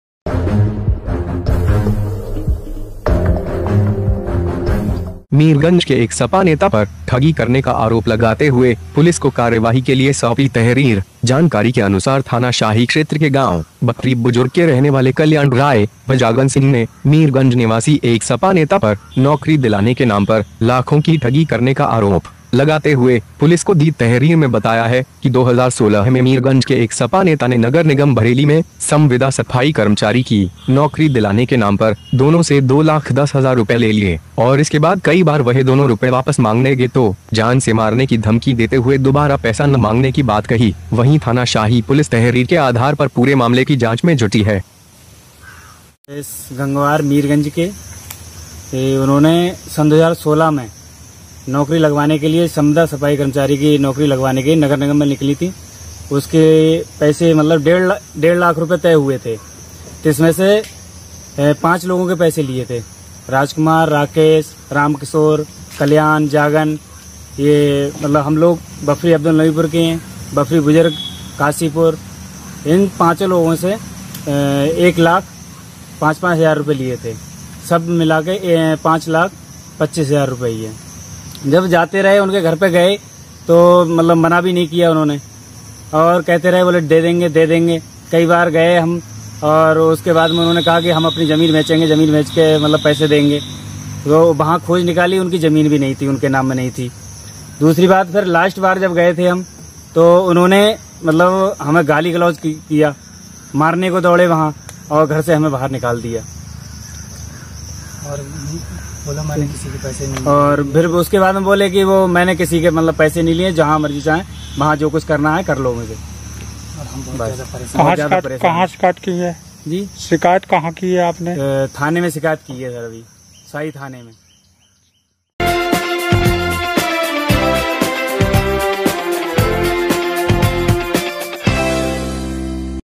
मीरगंज के सपा नेता पर नौकरी के नाम पर लाखों रुपए की ठगी करने का आरोप लगाते हुए पुलिस को सौंपी तहरीर
Bareilly News : मीरगंज के सपा नेता पर नौकरी के नाम पर लाखों रुपए की ठगी